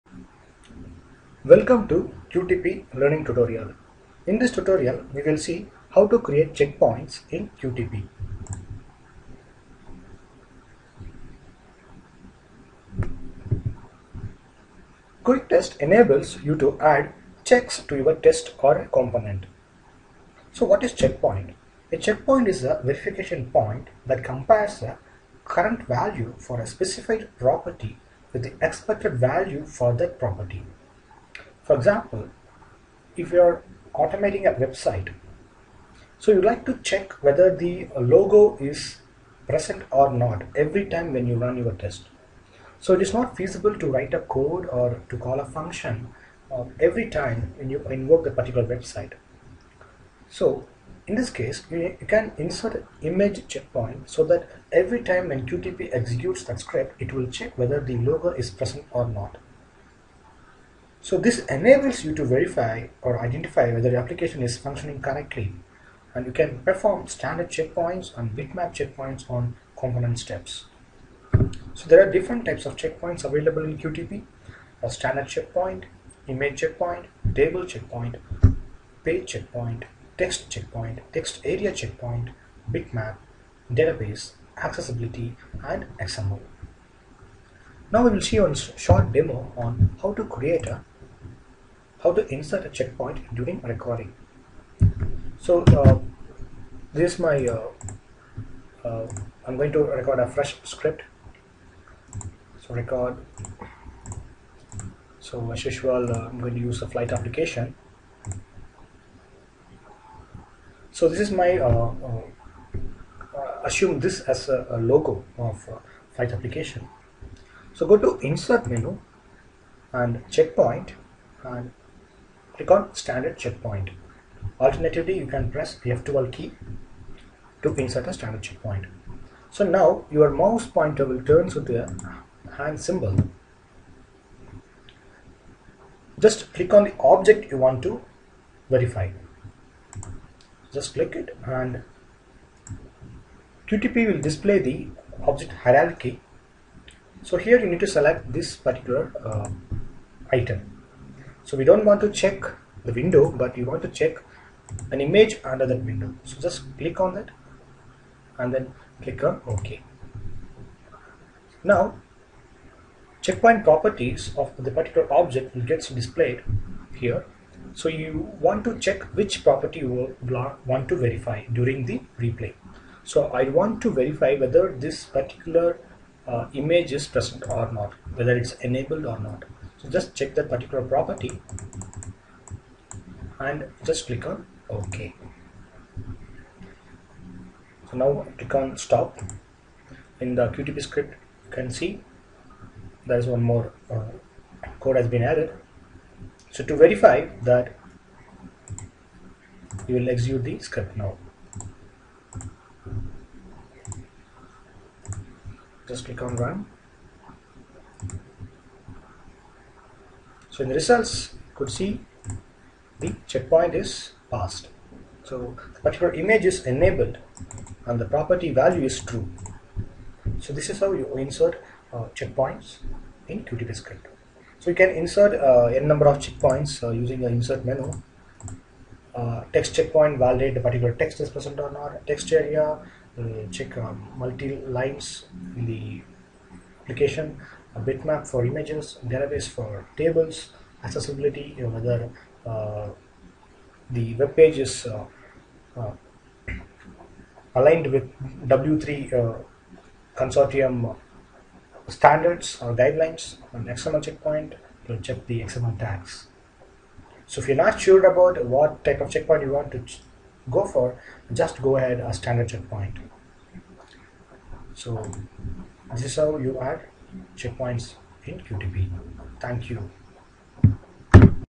Checkpoints in HP QTP (UFT)
This video demonstrates about various types of checkpoints in HP QuickTest Pro (QTP) aka Unified Functional Testing (UFT). for more tutorials.
Reduce your auto insurance rates by switching it to Root. Get $25 bonus now.